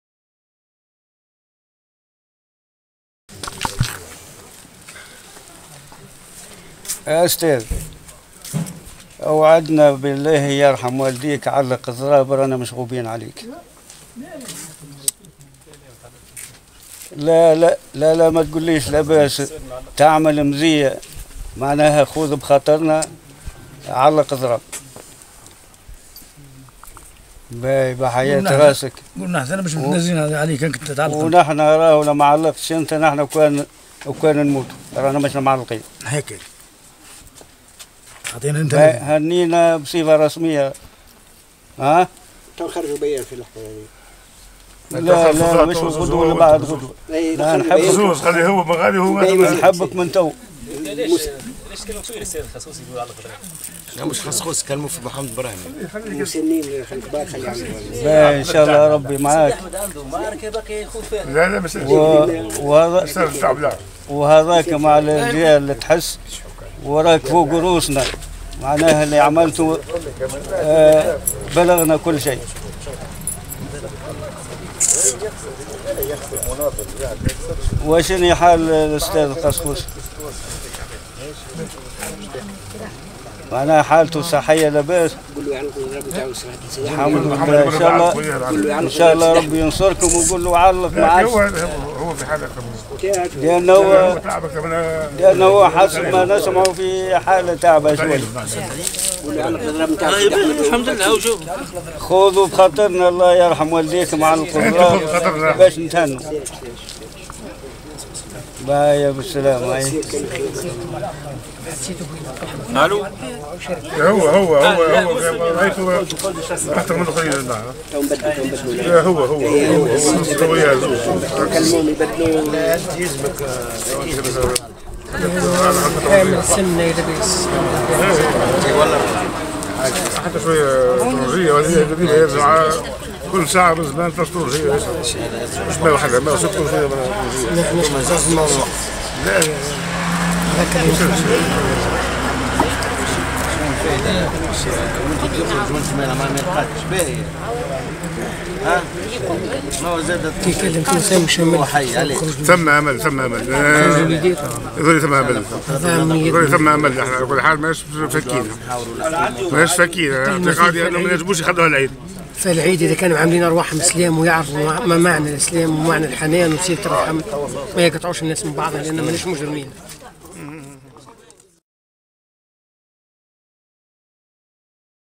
Village El Omrane
Sidi Bouzid - Village El Omrane
18 octobre 2012
Délégation d'El Massar au chevet des grévistes de la faim, réclamant la libération de leurs enfants